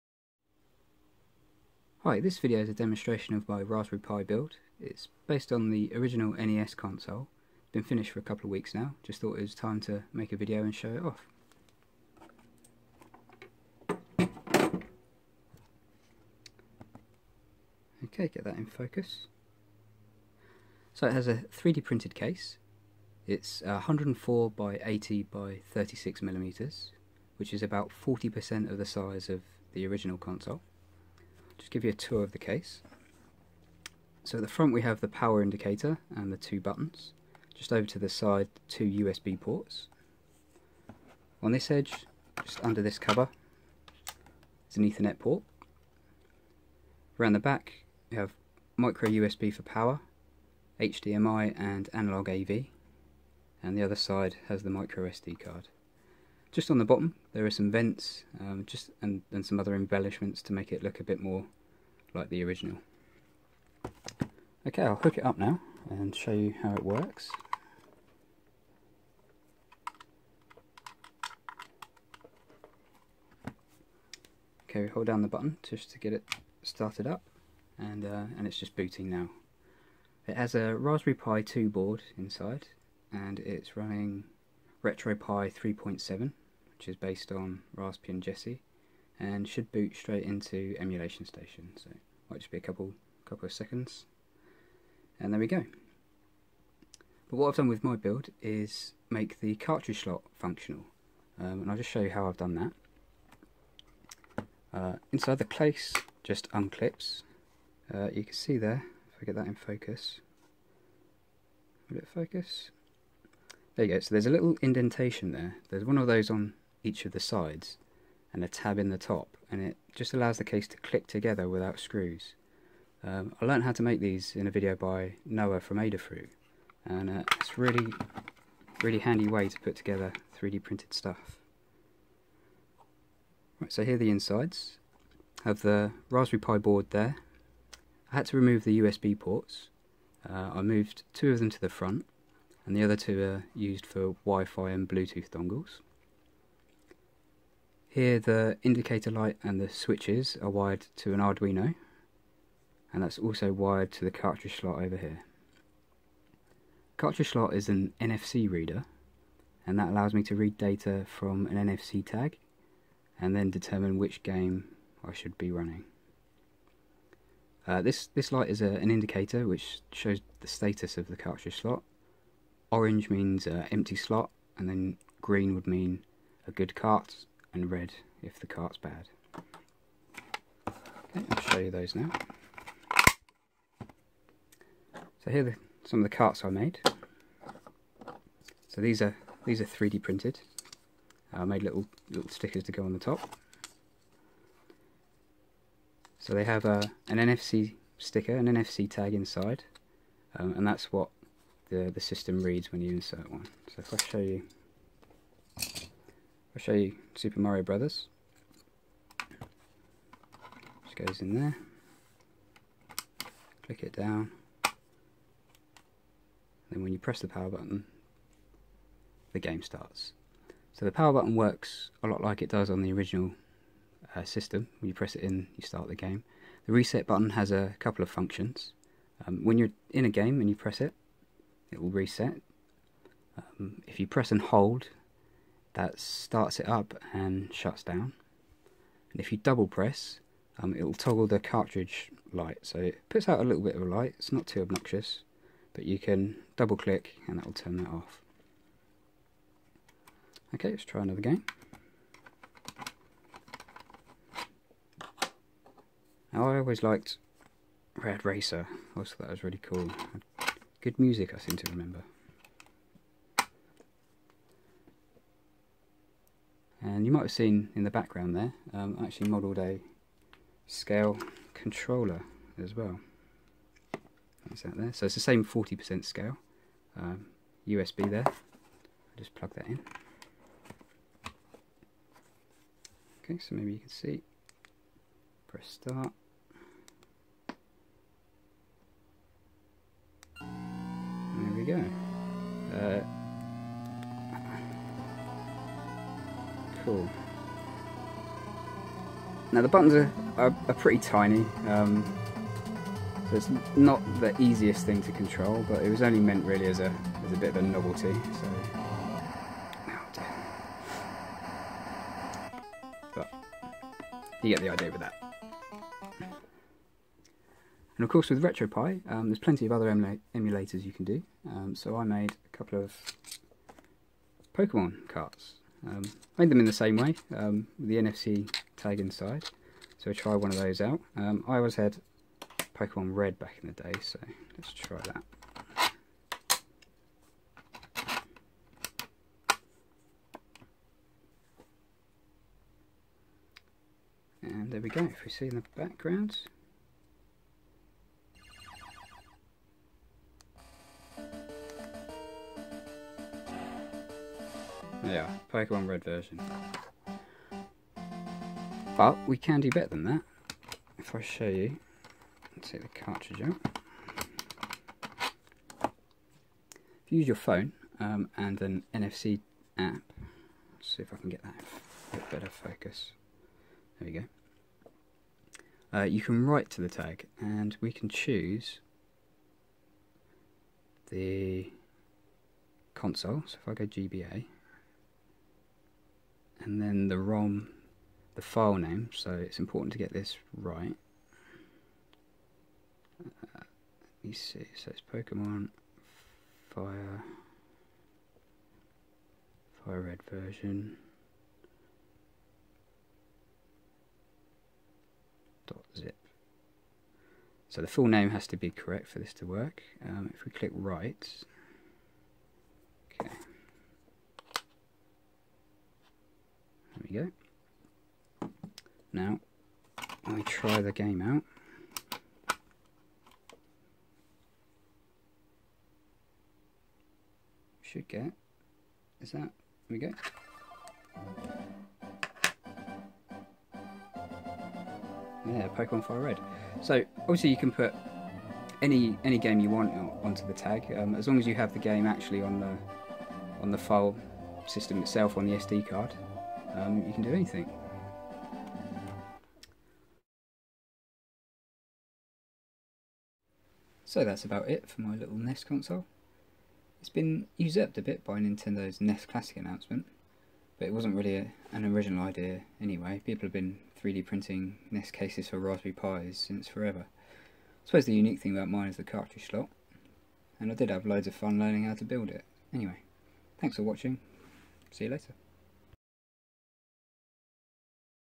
Raspberry Pi Mini NES Classic Console
This is a demo of my Raspberry Pi 'NES Classic' build.